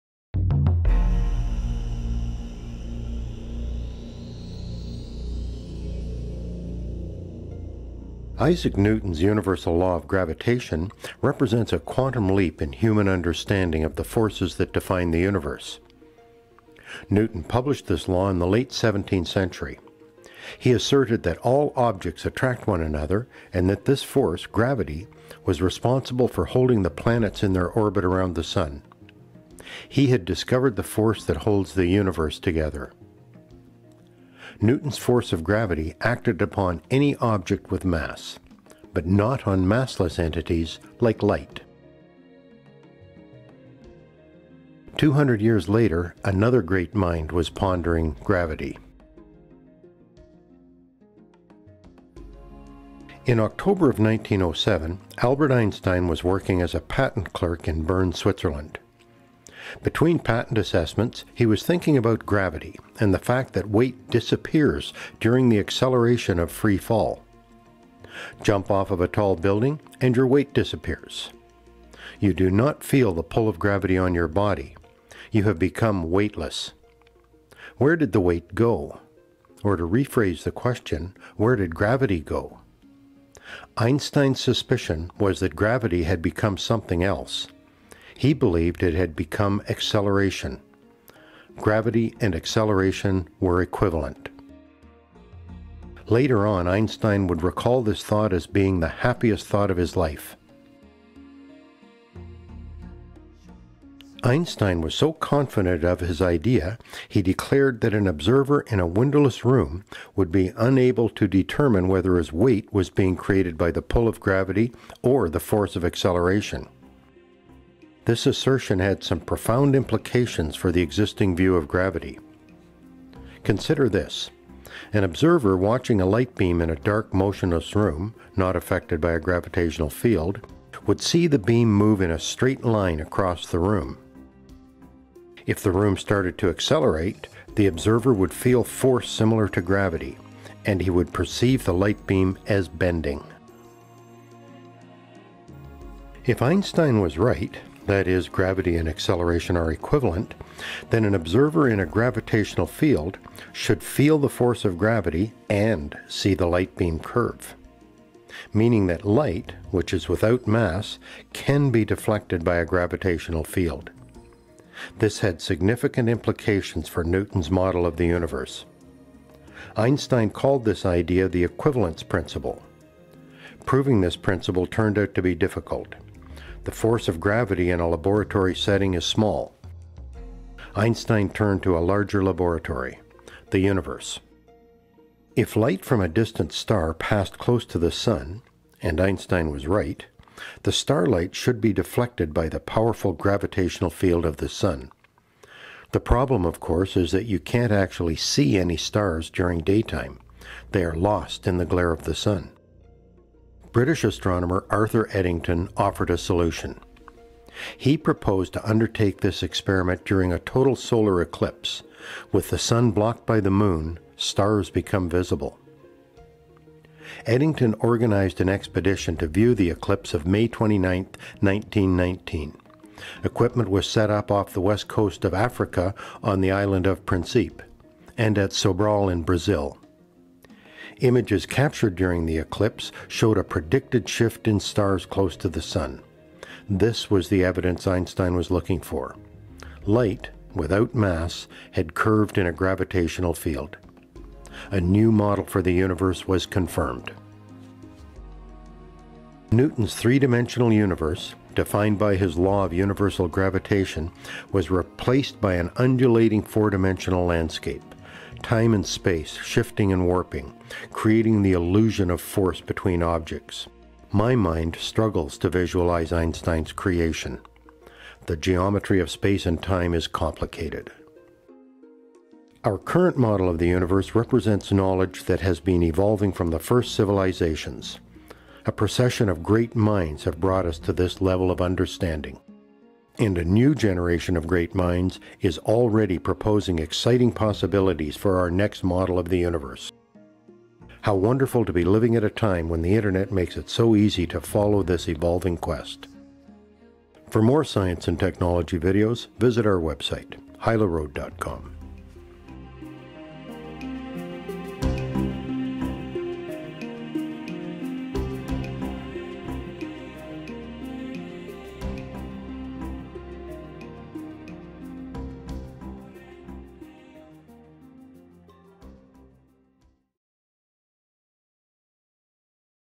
Redefining the Universe - Gravity and Acceleration
Einstein came to the  conclusion that gravity and acceleration were indistinguishable, this was a brilliant leap of imagination that redefined the universe.